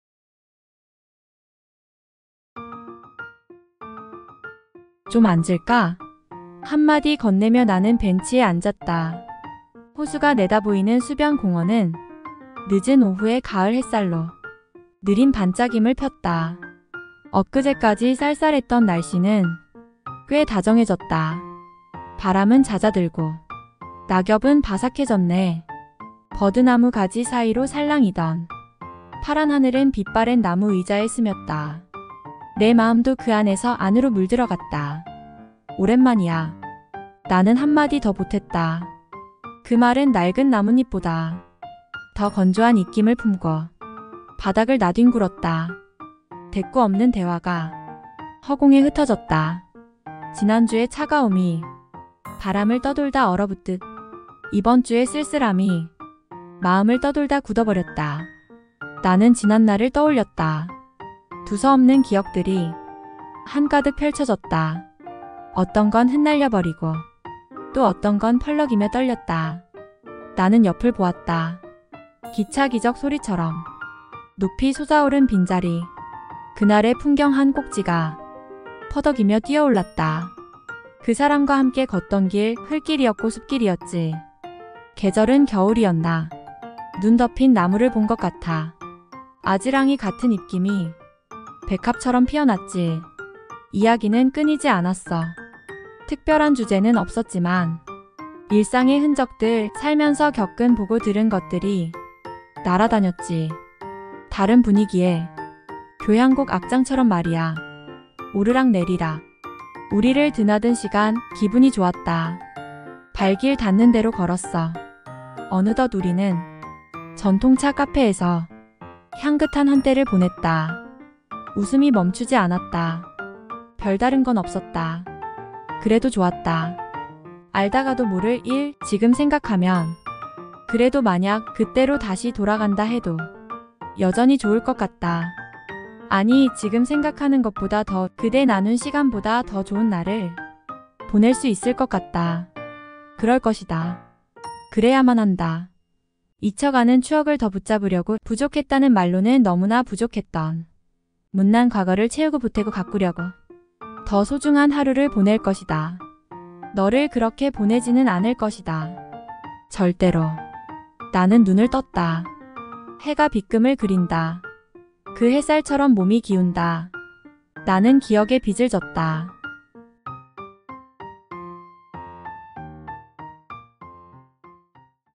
[ 11/8 - 에세이 ] '나는 기억에 빚을 졌다'
매일 브런치스토리에 올리는 글을
영상으로 만들었습니다.

『365일 작가연습』 이라는 책을 중심으로
매일의 주제에 맞춰 글을 쓰고 있습니다.
11/8일의 주제는 '나는 기억에 빚을 졌다' 입니다.
감사합니다.